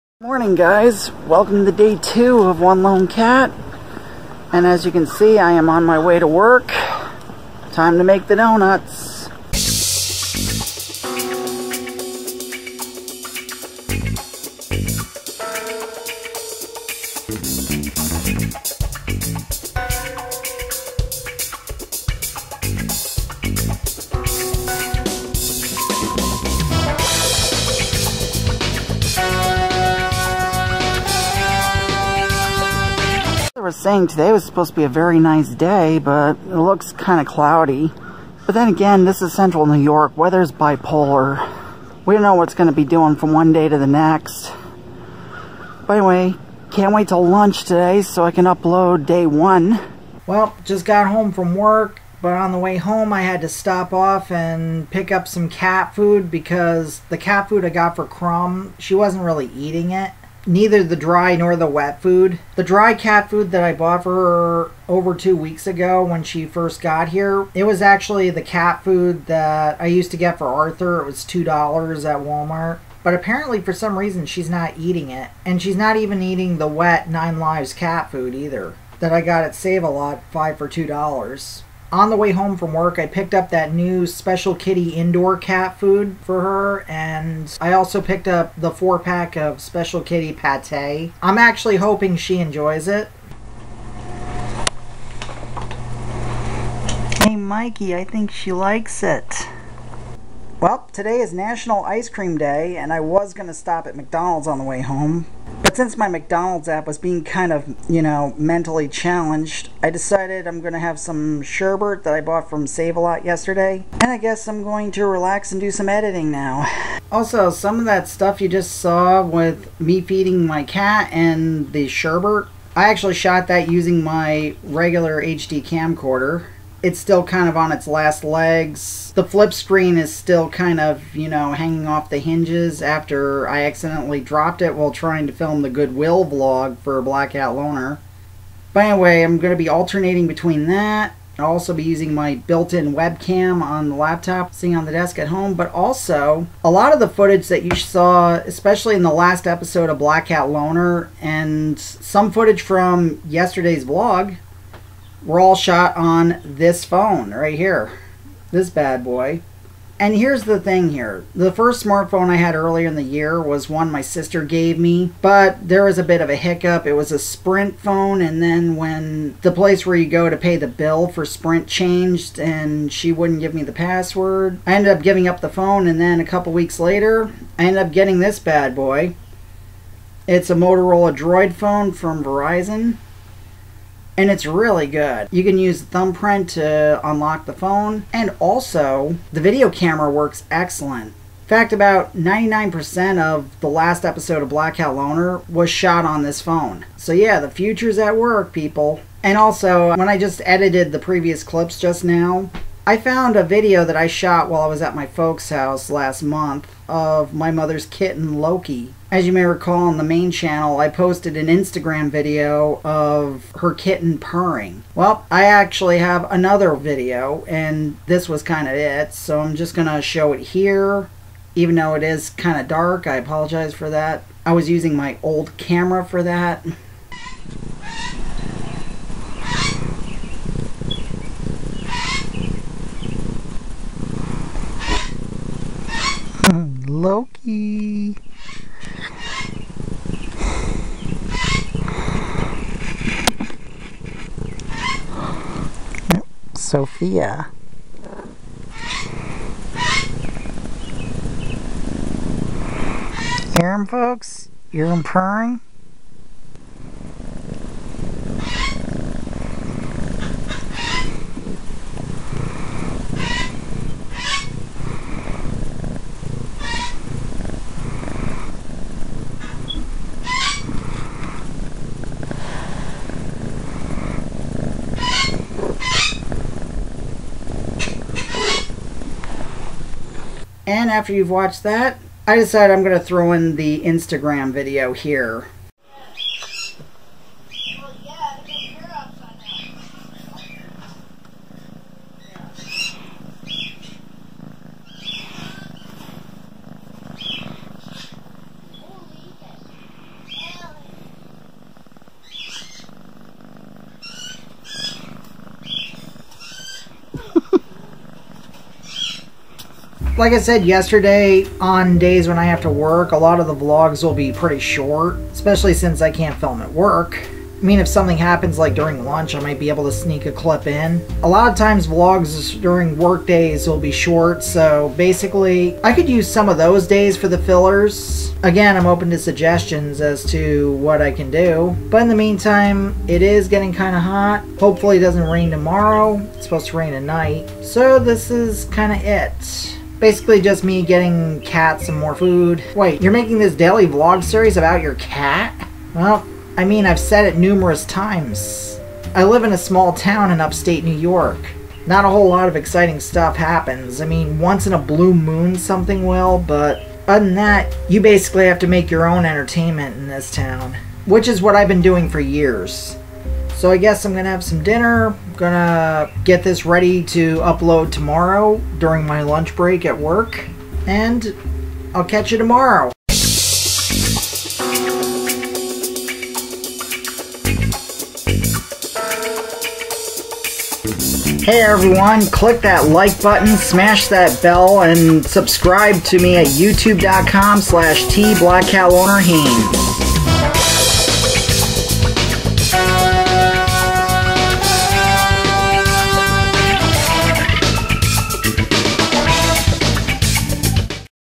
CAT FOOD NOW! (#2- 7/16/17)
I find out that my cat is not eating the cat food that I bought for her so I end up getting her some better food.